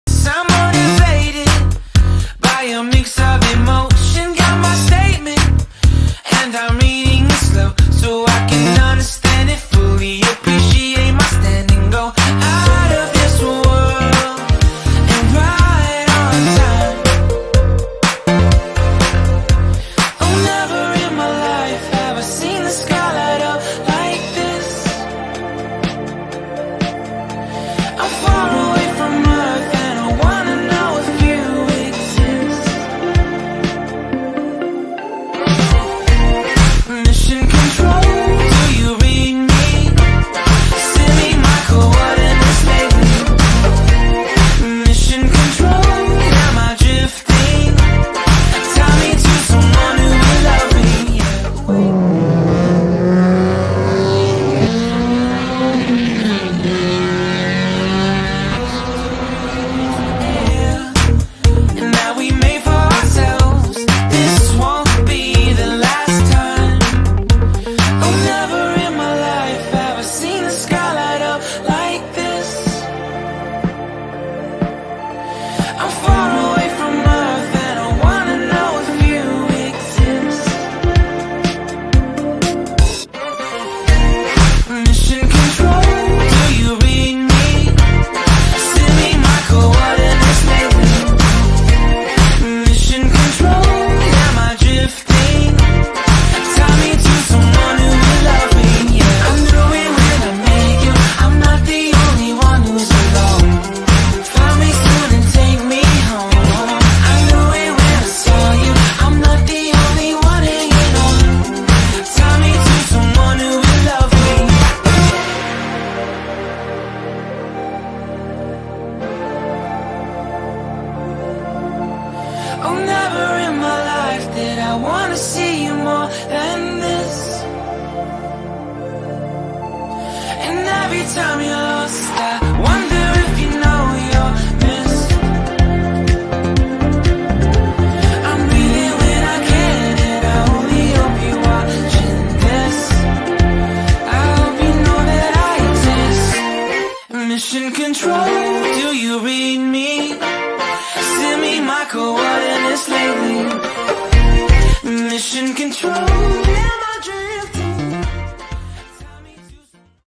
Pneu Furado no TrackDay da TRS Racing Team - Autódromo de Interlagos
Video TBT gravado antes da Pandemia (COVID-19).
Acompanhe um pouco do que rolou no último TrackDay da TRS Racing Team.
Deu Ruim pro Sandero RS